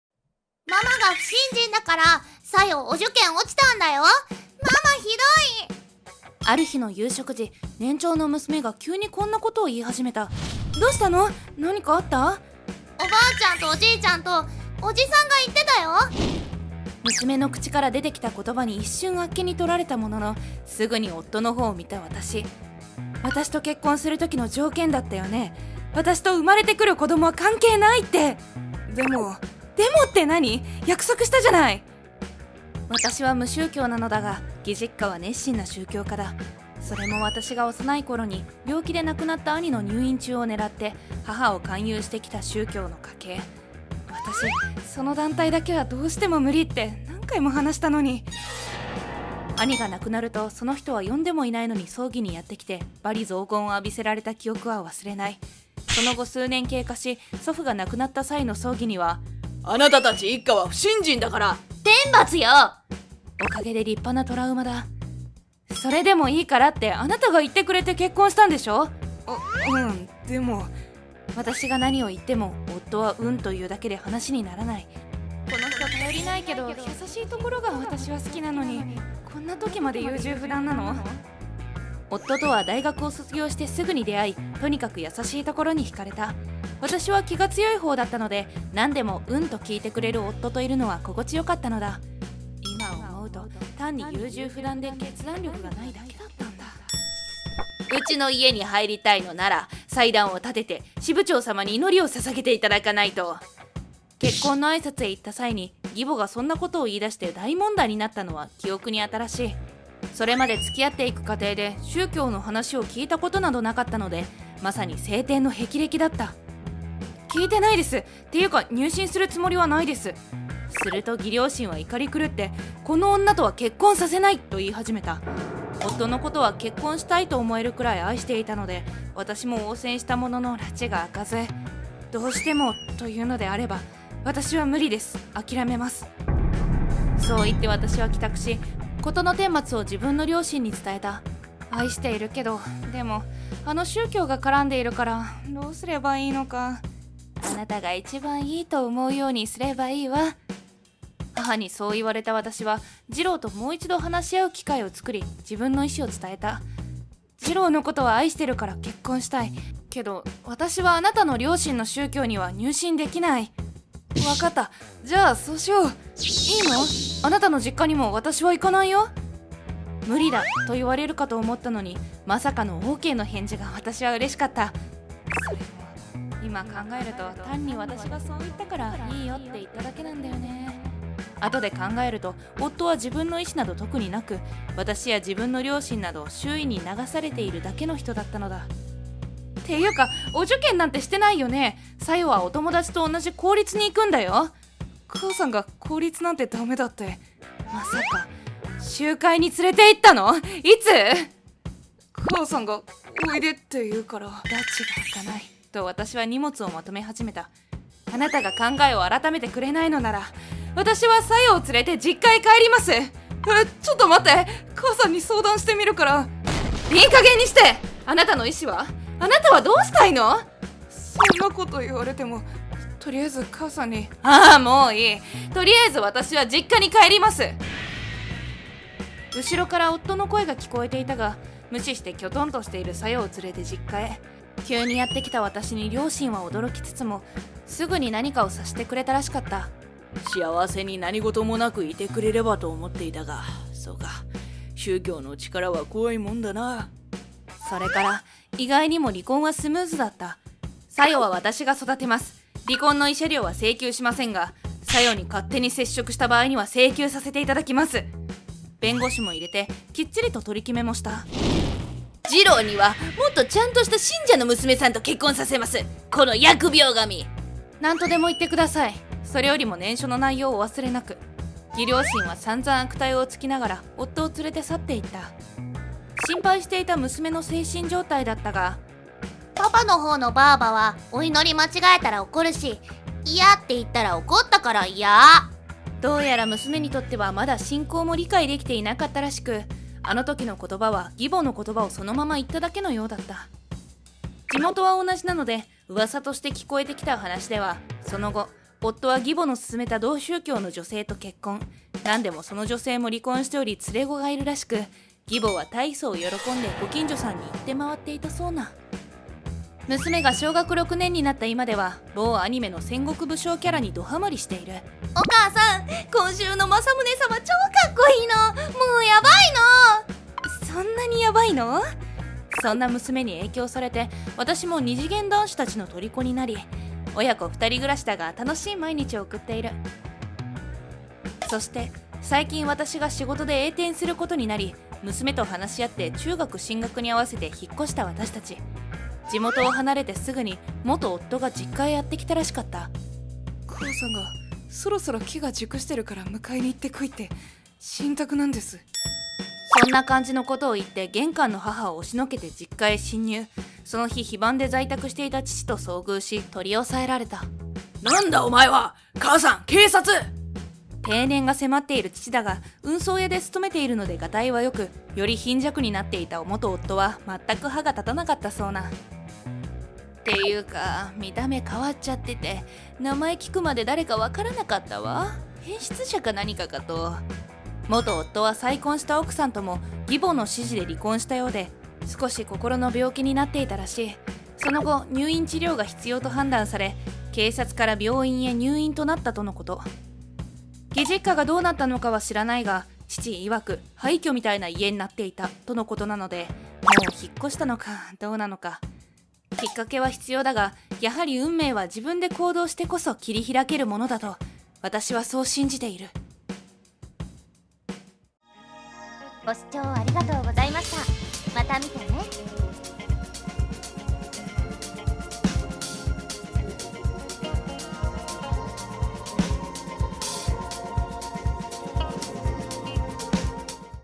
【漫画】夫「お前も子供も関わらせないから結婚してくれ」私「おｋ」婚約の挨拶に行ったら義実家は熱心な宗教家だった→ある日娘がその宗教の言葉を使うようになり私を責め始め・・・（スカッと漫画）【マンガ動画】
〜あらすじ〜
旦那の実家は熱心な宗教家。私は入信しないと結婚する前にあれほど言ったのに娘を宗会に連れて行かれ...